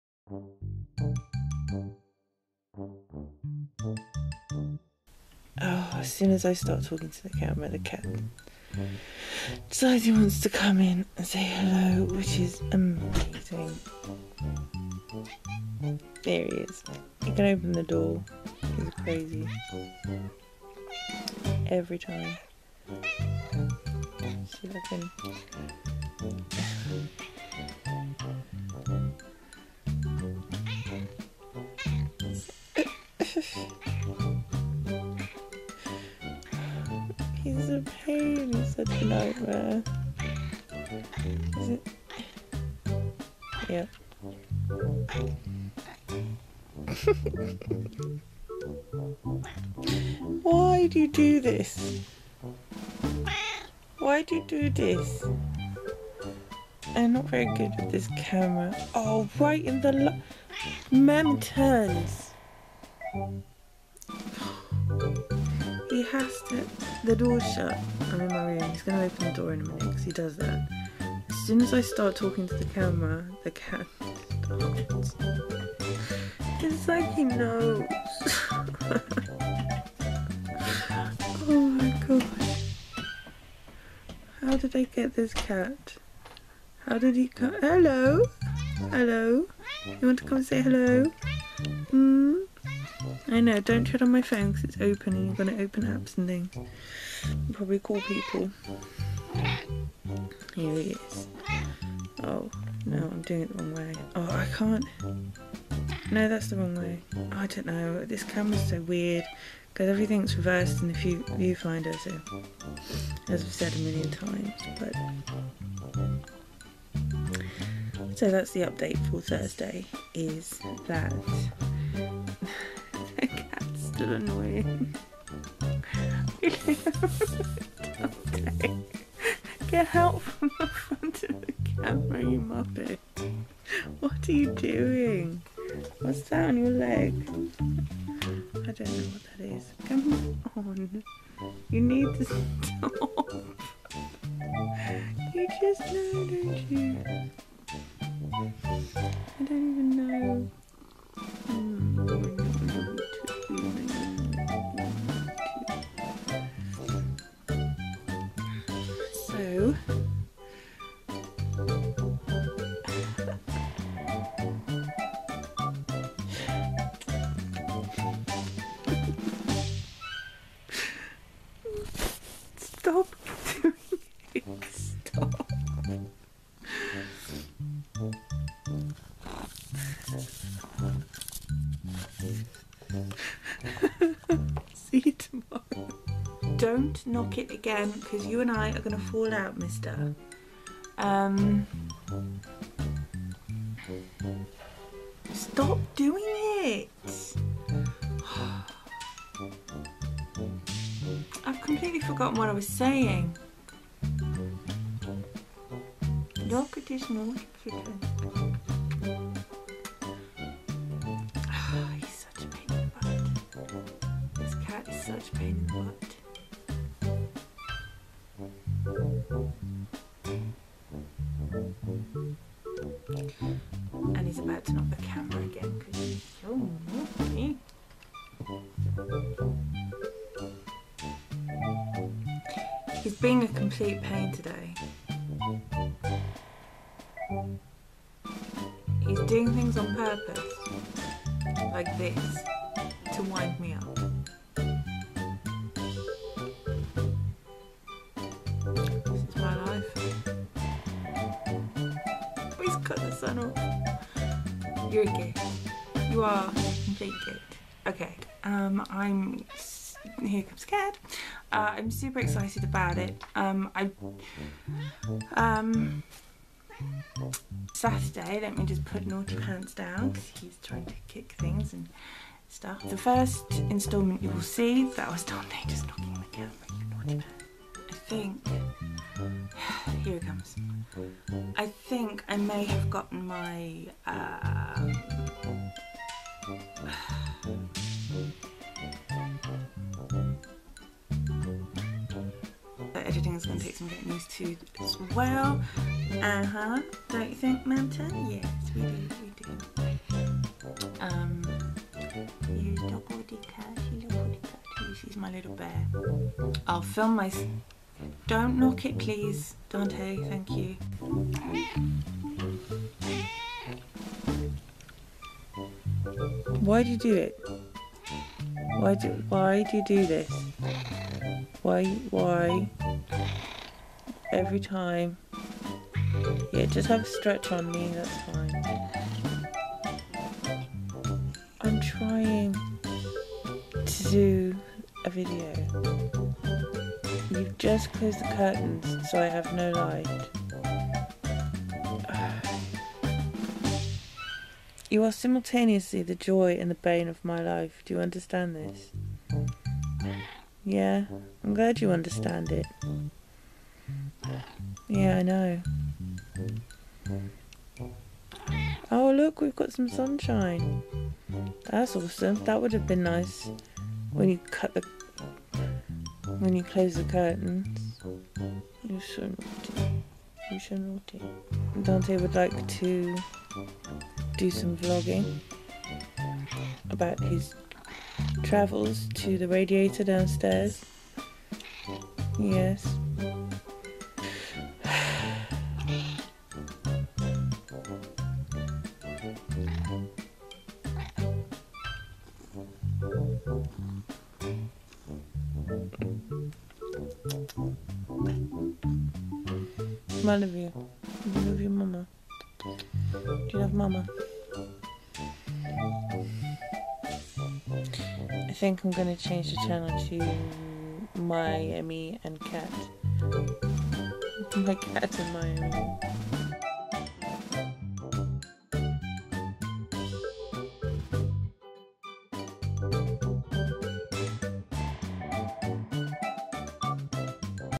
meowttakes | funny vlog cat outtakes | chronic illness vlog outtakes
Hey my lovelies! Well here it is as promised some of the 'meowttakes' from the weekly vlog, i hope you find them as funny as i did, i had so much fun making this so let me know if you want this again!
I thought maybe a bit of background on Dante may be appropriate, he was a rescue who had been taken from his mother far too early and then abused, he never learned how to play and sometimes has trouble being a 'cat'. I'm pretty sure he's an actual baby! He has an eating disorder and very bad attachment issues but as I'm home all the time i can give him all the attention he needs and deserves. I may get a little exasperated sometimes but i wouldn't swap him for anything and he makes me smile every day.
So anyway sit back and enjoy, and i hope you guys are all doing as well as can be, wishing lots of love to you all from Dante and me! 💖